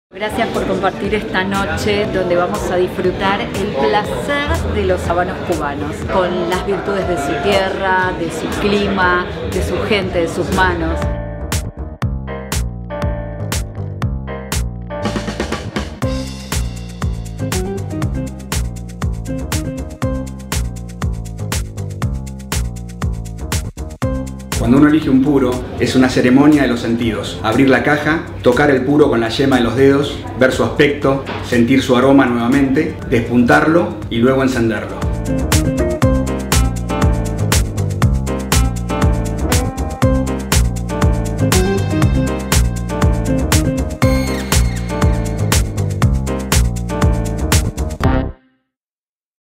Cohiba Atmosphere Experience
Un encuentro diseñado para conocer más sobre el mundo Cohiba donde Valeria Rey, sommelier de habanos, dio una charla y tips básicos sobre cómo elegirlos, conservarlos, experimentar sus aromas y sabores.